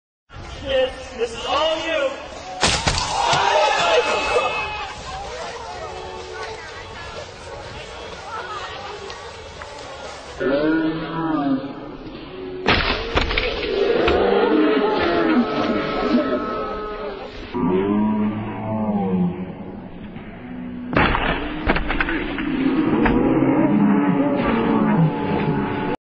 Ski Jump Fail
This guy must have broken a foot when he doesn't quite make the gap.